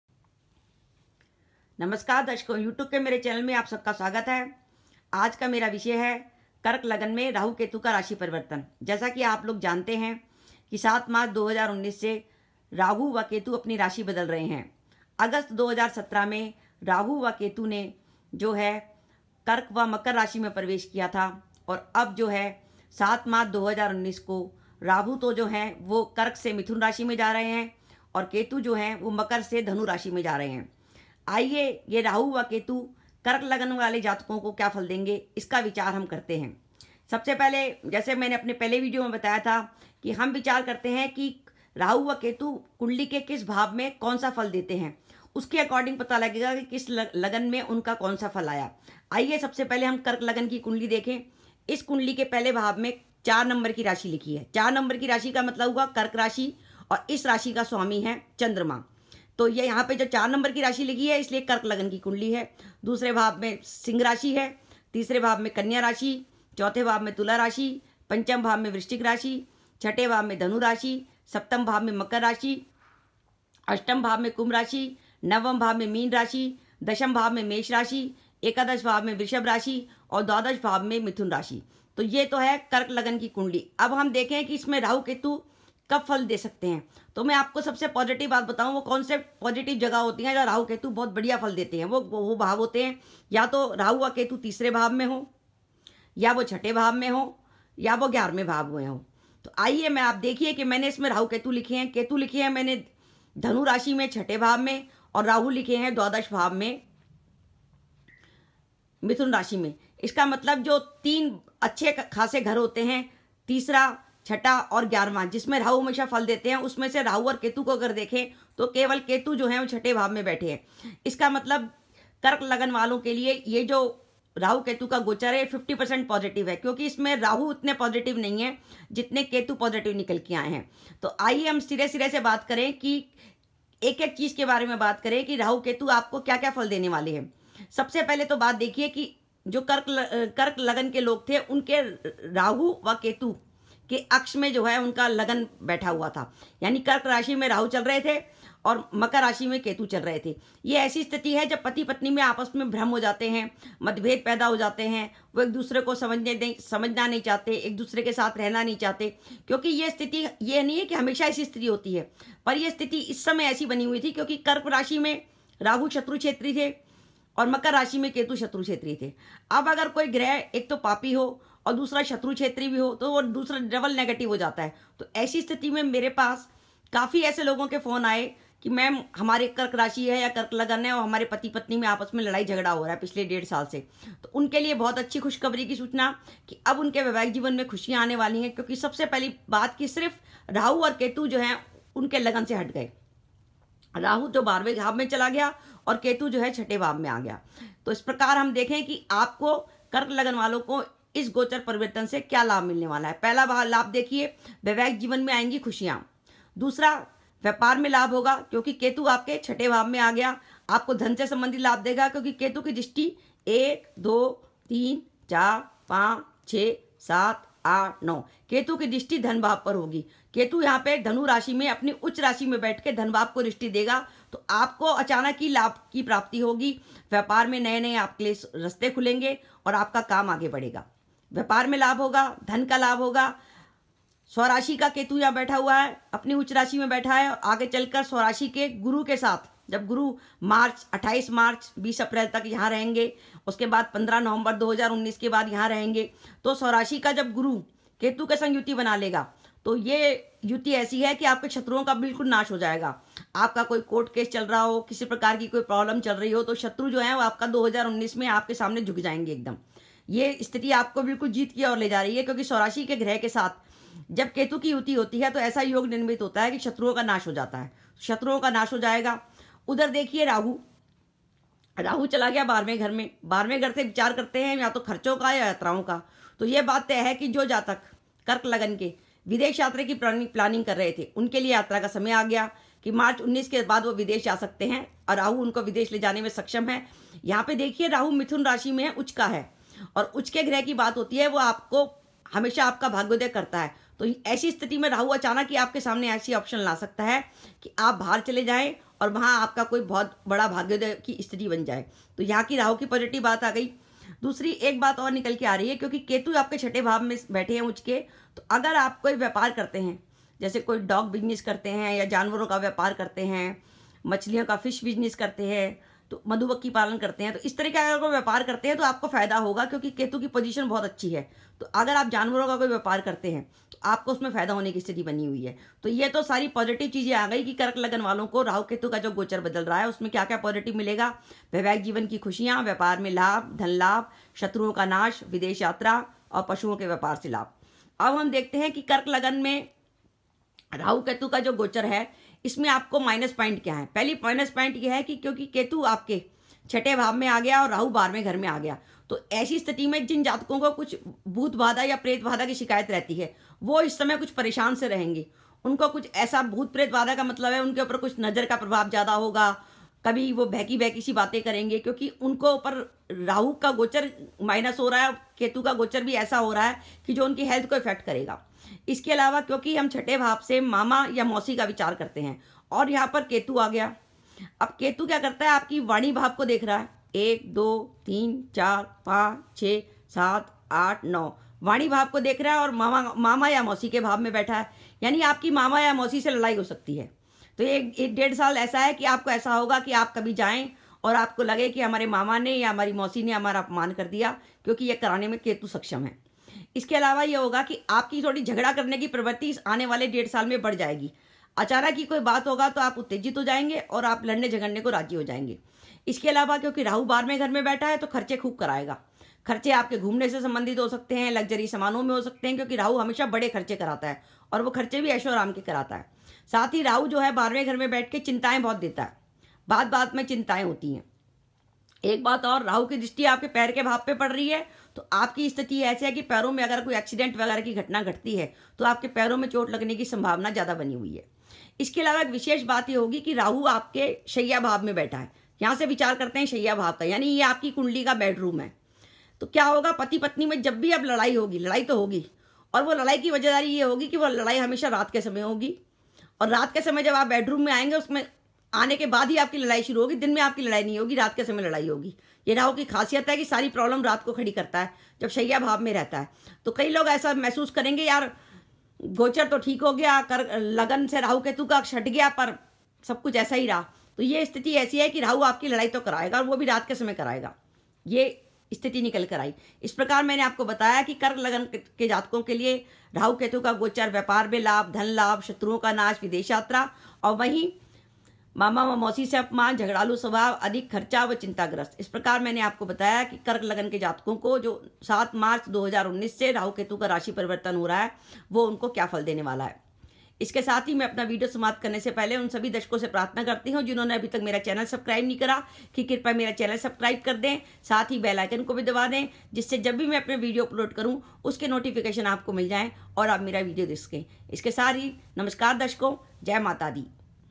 कर्क लग्न मे राहु व केतु का राशि परिवर्तन 7 मार्च 2019 # धन लाभ शत्रुओं का नाश विदेश यात्रा
कर्क लग्न मे दामपत्य जीवन बढ़िया होगा |